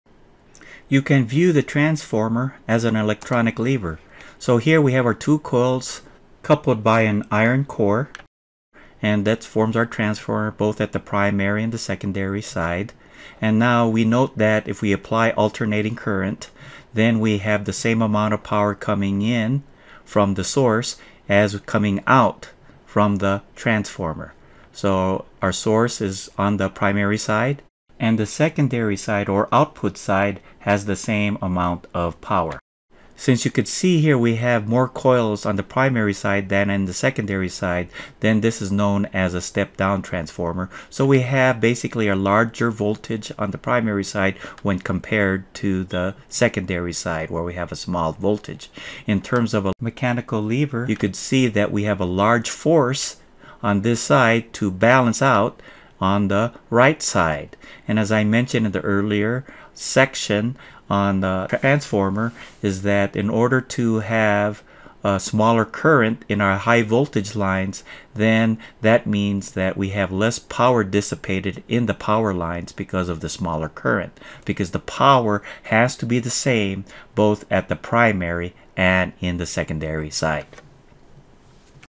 Transformers: An Electronic Lever
A transformer can be viewed as an electronic lever where the power at the primary and secondary must be the same.  So a smaller voltage at the secondary when compared to the primary implies a larger current in the secondary when compared to the primary.